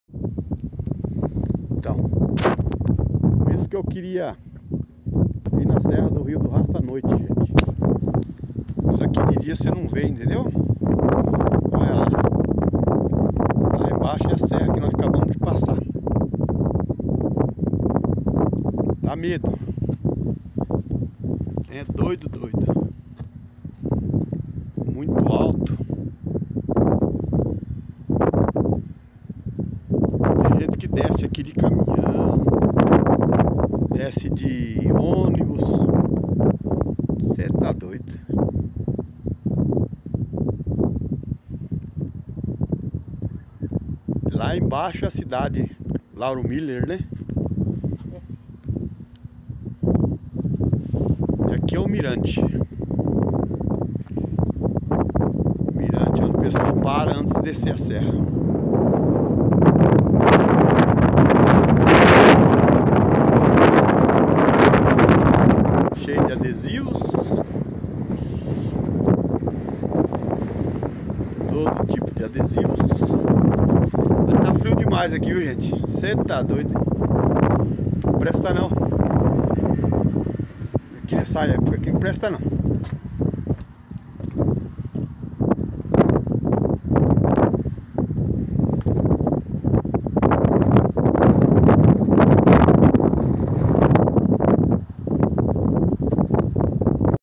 serra do rio do rastro a noite
fizemos  uma  viagem  de  carro  a  Santa  Catarina  para  conhecer  as  serras  catarinences